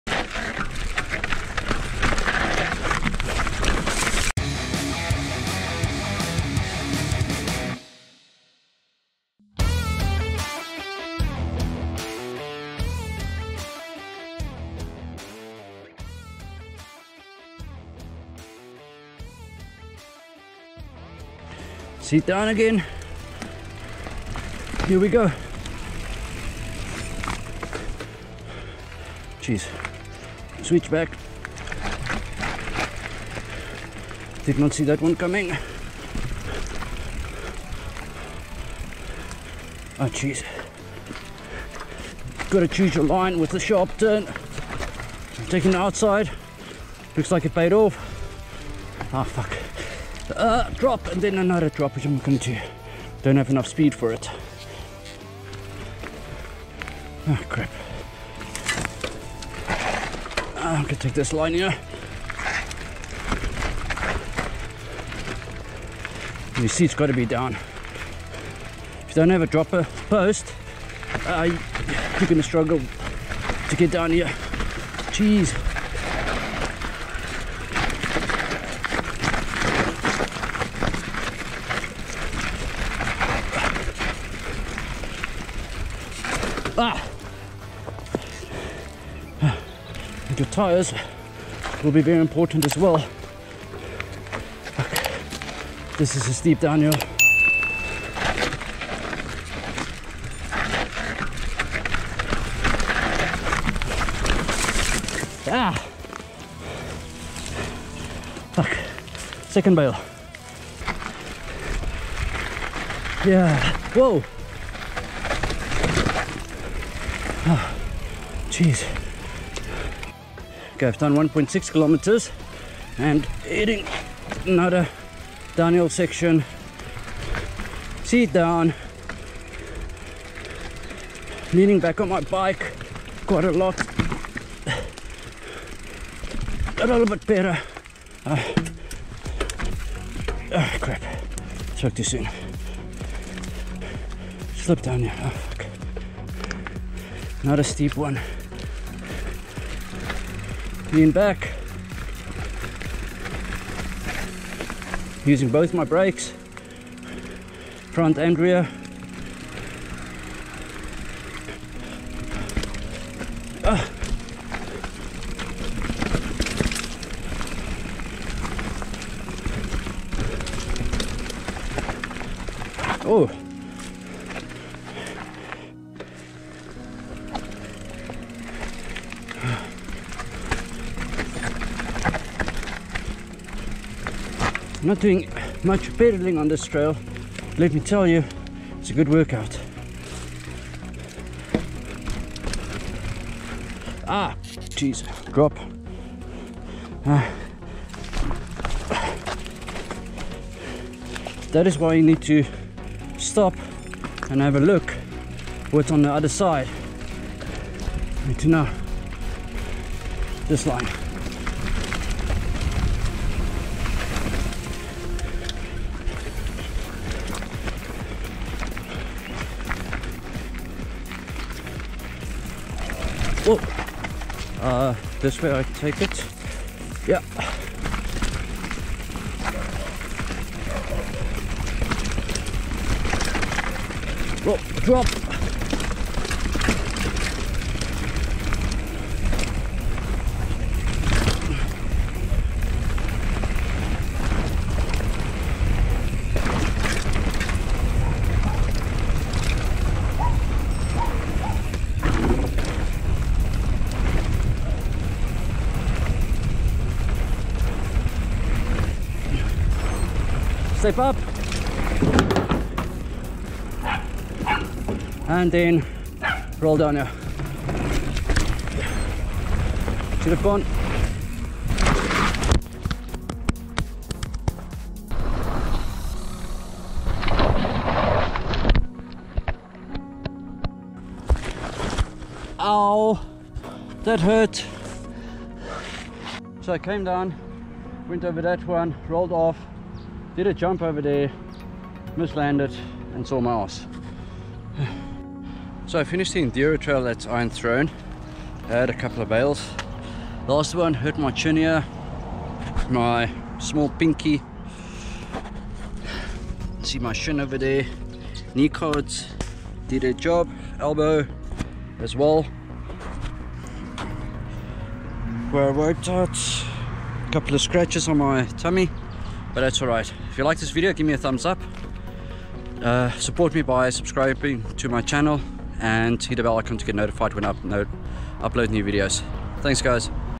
Watch me Bail! Iron Throne DH Park
Part 2 of the Kowyn Downhill Enduro Trail - I bailed a couple of more times, will I ever learn???